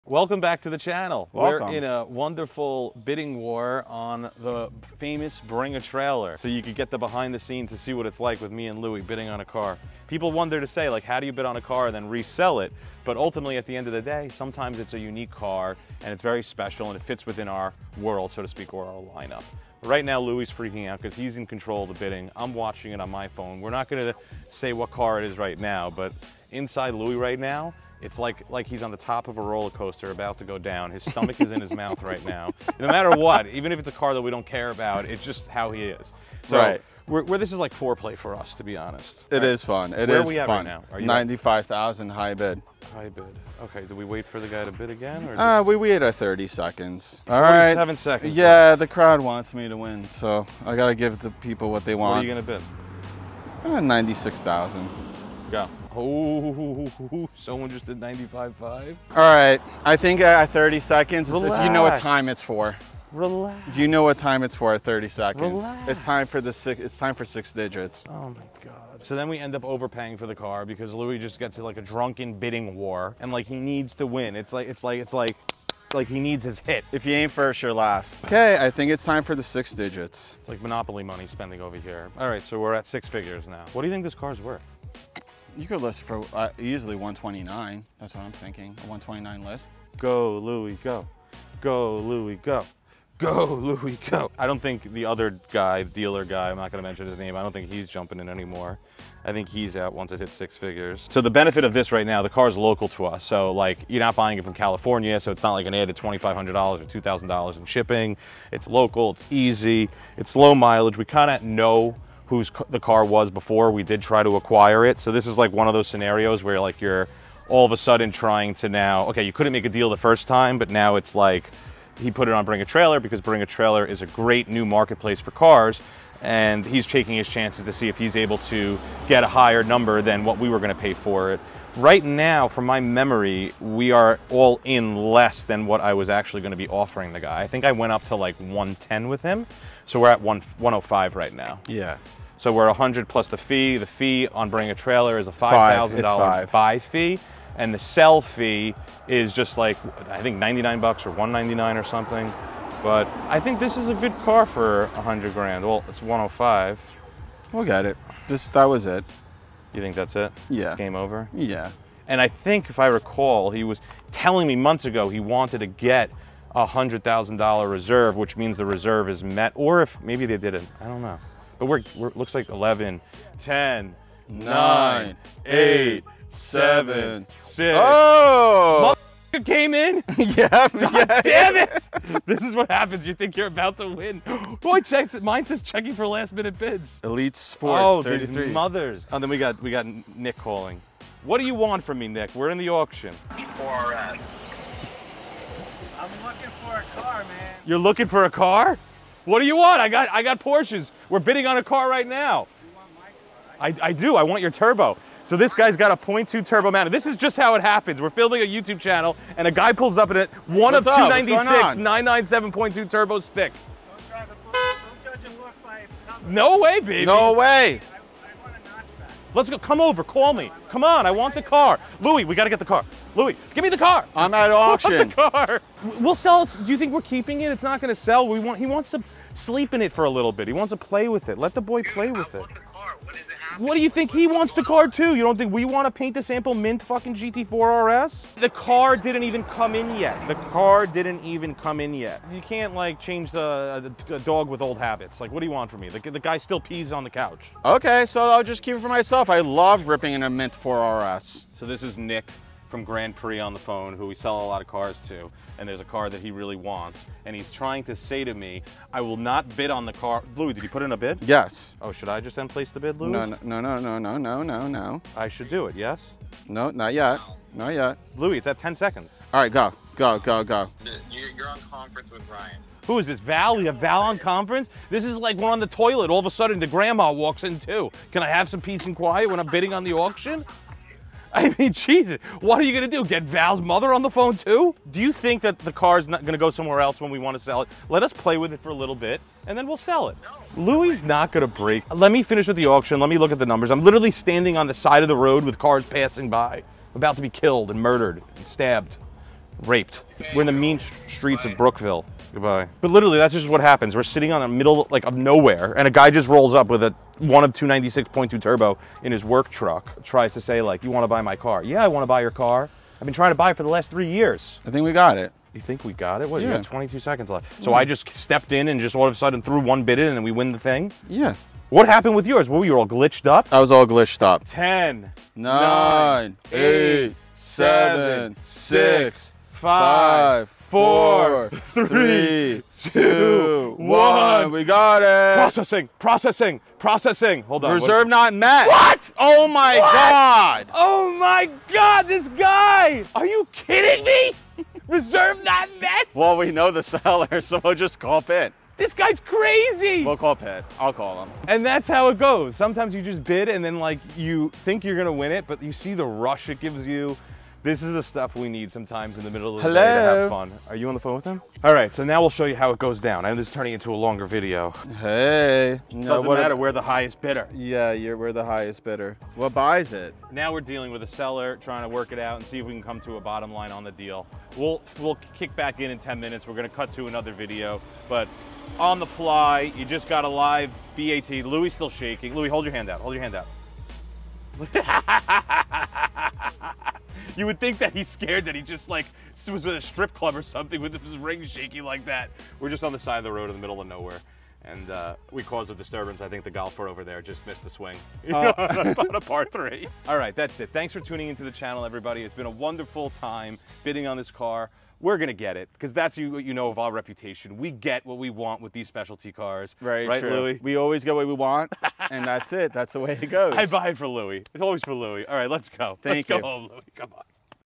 How to Bid on Bring a Trailer: The RFMC Way!
How Louie and Ryan bid on cars for inventory!

Cars for Collectors from Collectors

Placing the BEST of the BEST Porsches back on the assembly line and serving them to the world!